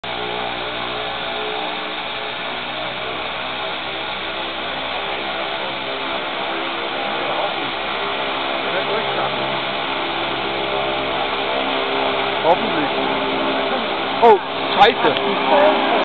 Only Footage of the Lukla (Nepal) Crash
This is the only video of the Lukla catastrophe, filmed by a German family from Stuttgart. All people, excep for the pilot, died. Most of them Germans